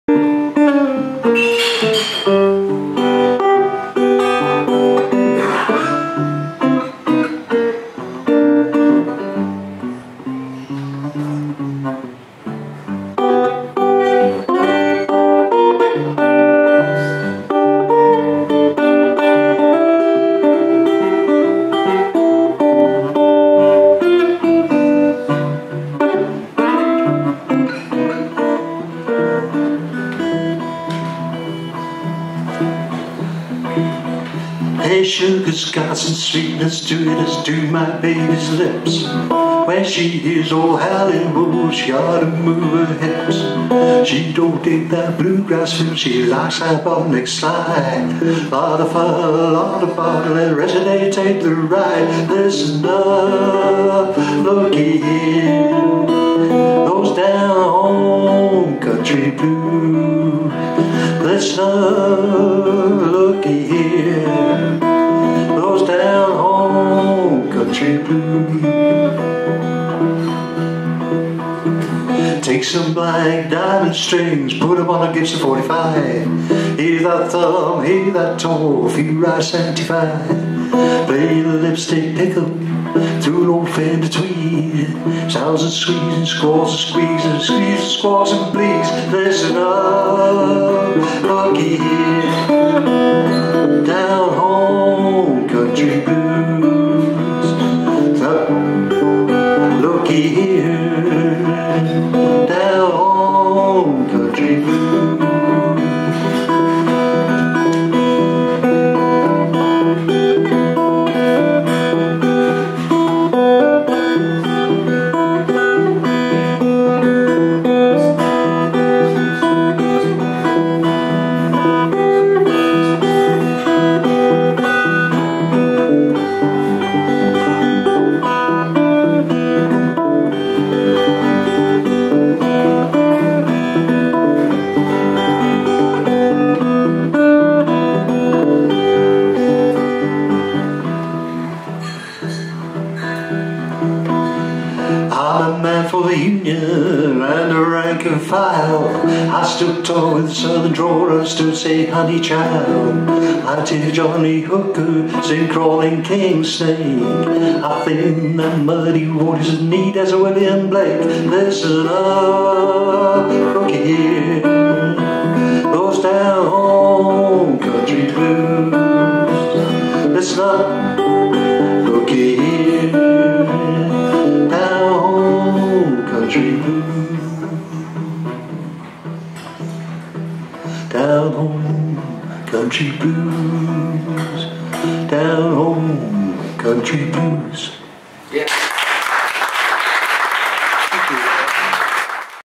Underground Blues May 2022 Ollie Holroyd.
Olie has a unique style love his finger picking and delivery of his music Open Mic May 2022.